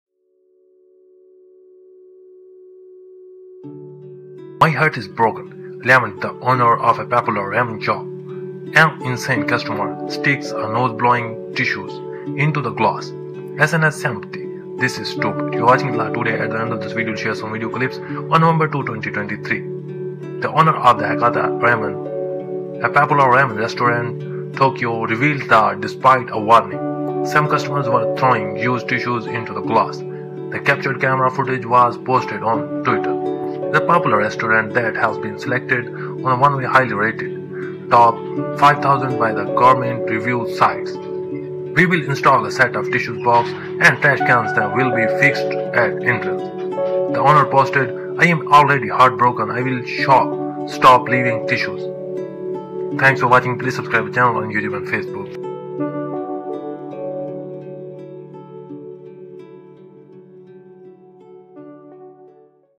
「心折れました」人気ラーメン店主嘆き 鼻かみティッシュをグラスに突っ込む非常識客...SNS同情「これはアカン」
「心折れました」人気ラーメン店主嘆き　鼻かみティッシュをグラスに突っ込む非常識客...SNS同情「これはアカン」``My heart is broken,'' laments the owner of a popular ramen shop. An insane customer sticks a nose-blowing tissue into the glass... SNS sympathy: ``This is stupid.''
東京・高田馬場にある人気ラーメン店「博多ラーメンでぶちゃん」itlatodaynews 店主が2023年11月2日、注意書きがあるにもかかわらず、使用したティッシュをグラスに捨てる客がいたとして、その#world 一部始終を捉えたカメラ映像をX（ツイッター）japannewstoday で公開した。video 【実際の写真】鼻をかんだティッシュが突っ込まれたグラス 　同店はグルメレビューサイト「食べログ」で評価の高い#itlatodayworldnews 「TOP5000」にも選ばれている人気店だ。 ■「入口に固定するティッシュ箱とゴミ箱をセットで設置します」 　店主は投稿で、「もうね 心が折れましてね ティッシュ置くのやめます...worldnews .注意書きしてあっても ティッシュを置いてる我々が悪い」と切り出し、店内のカウンターを写したカメラ映像を公開した。